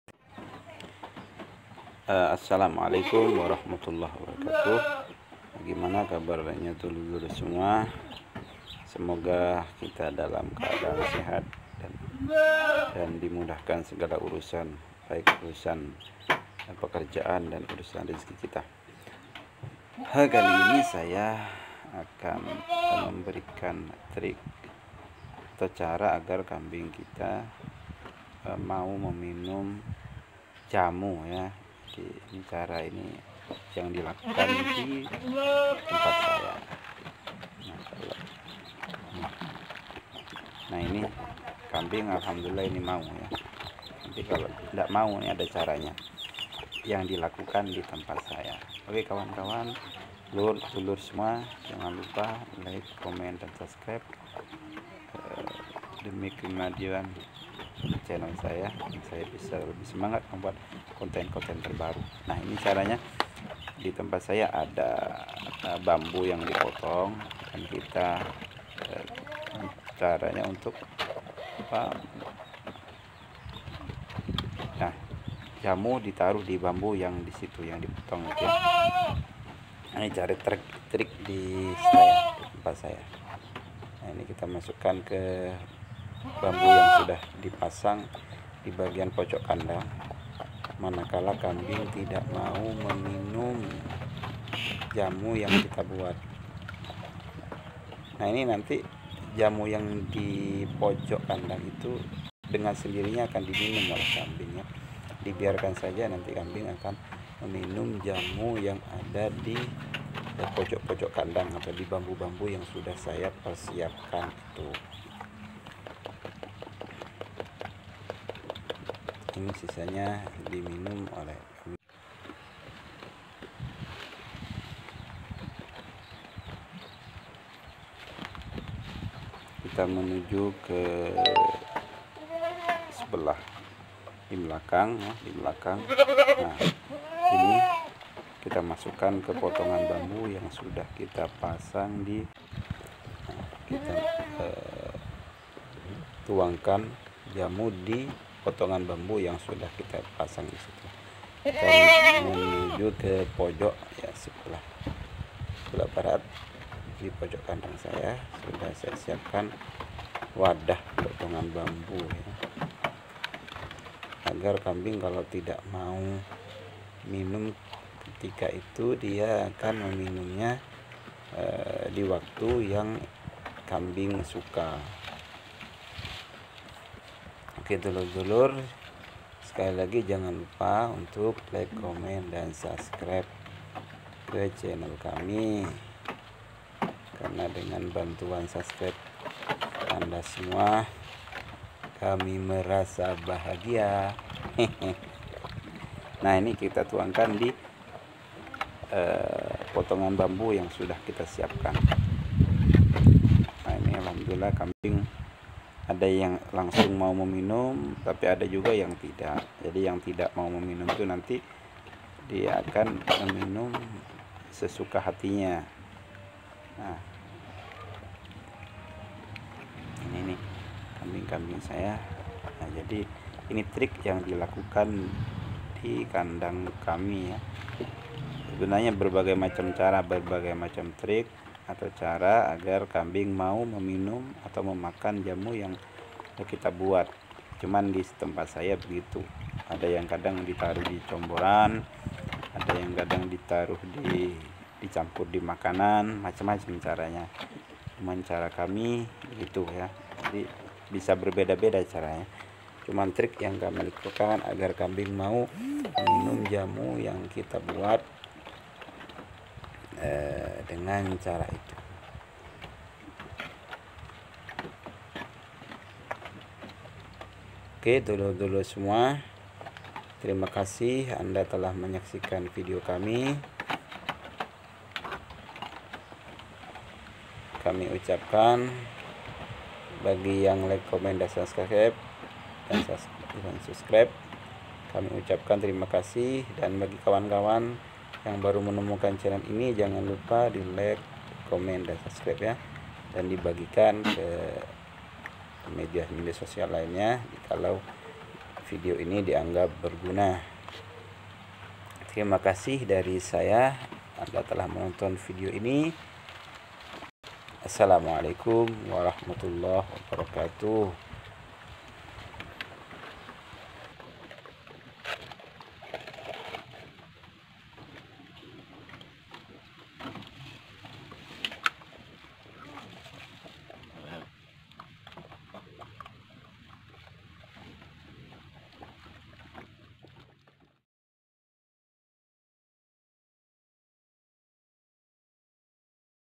Cara jitu agar kambing mau minum jamu
Cara jitu agar kambing mau minum jamu cara yang kami lakukan dengan menuang kepotongan bambu yang kami pasang di pojok-pojok kandang ini salah satu cara yang kami lakukan